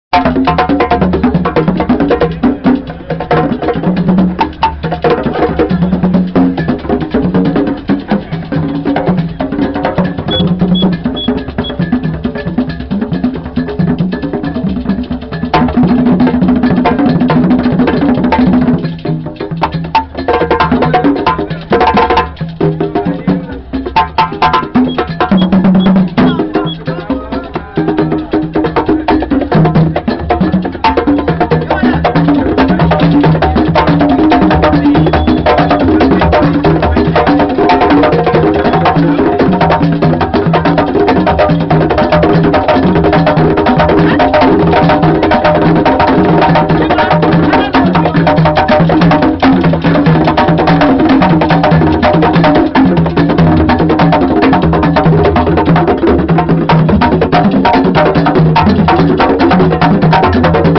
Jamming with Kakatshitshi
Kakatshitshi Master Drummers from Ghana performing grooves from West Africa on a sunny day by the Thames in London - surrounded by thousands of people protesting about world poverty. Yaw Asumadu on kpanlogos and djembes.